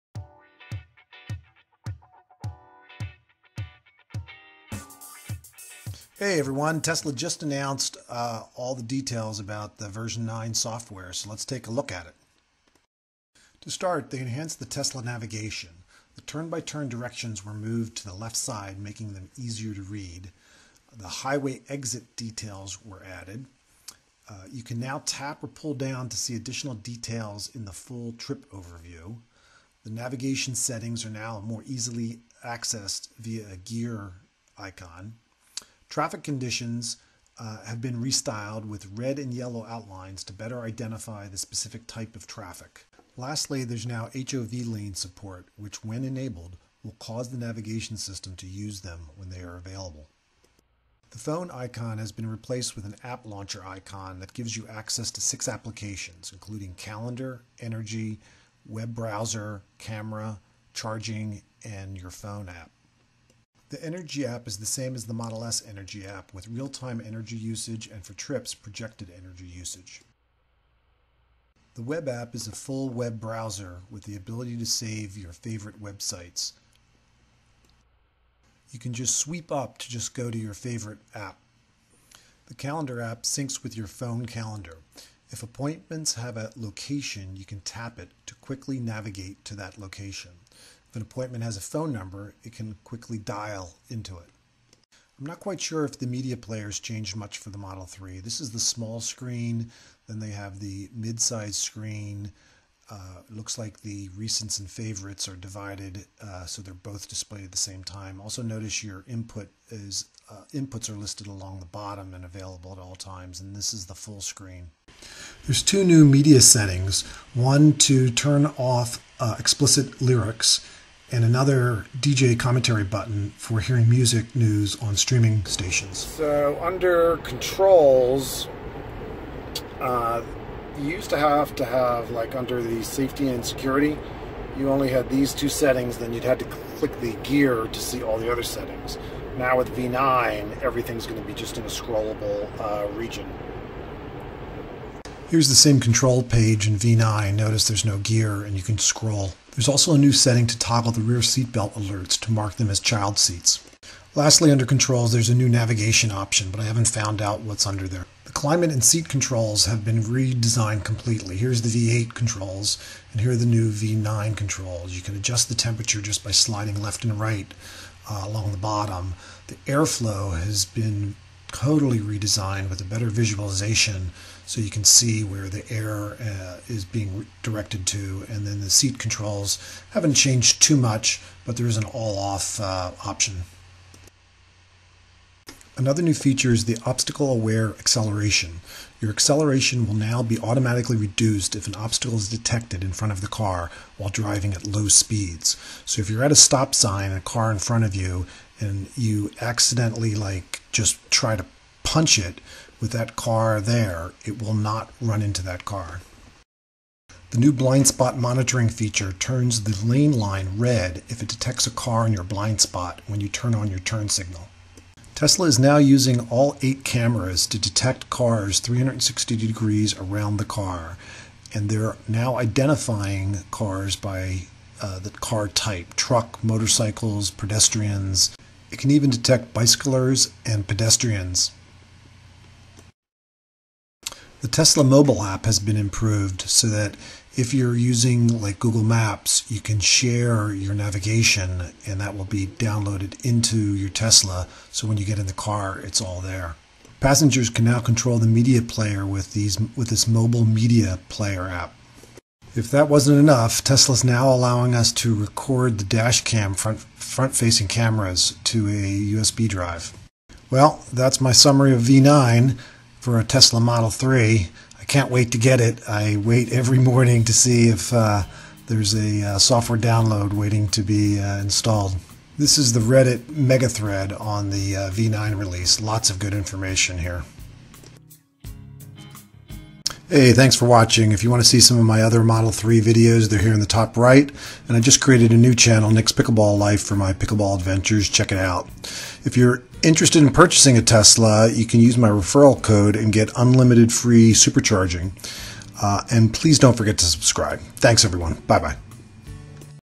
Tesla V9 Was Released!!!!!
Day 204 - Tesla officially released V9 so I did a deep dive into all the new features for the Model 3.

Current Software Version: v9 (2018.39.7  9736c9b)